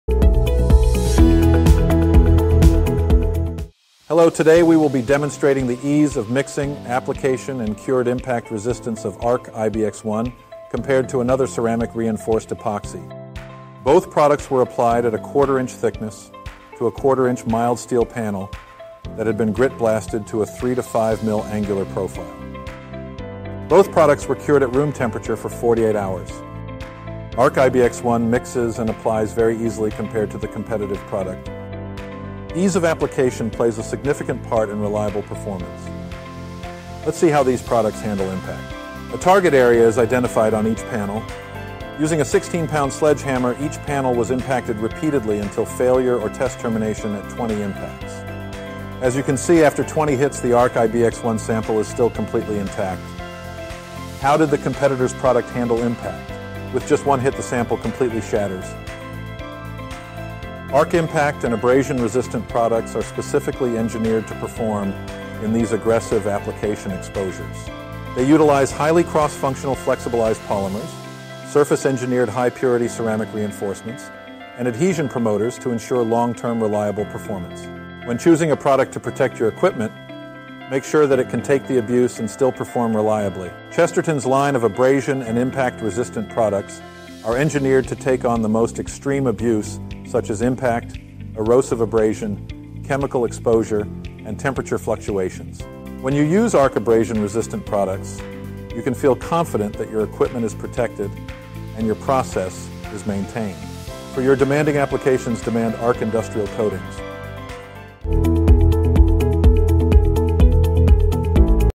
Sledgehammer Impact Resistance Test: ARC I BX1 Coating vs Competition
Chesterton’s ARC I BX1 is a 100% solids, ceramic reinforced modified epoxy, with enhanced impact, and abrasion resistance coating. Easily applied by trowel, ARC I BX1 provides superior abrasion resistance while also having the ability to withstand severe impact. Use ARC I BX1 to increase equipment life and reduce downtime in your most demanding applications.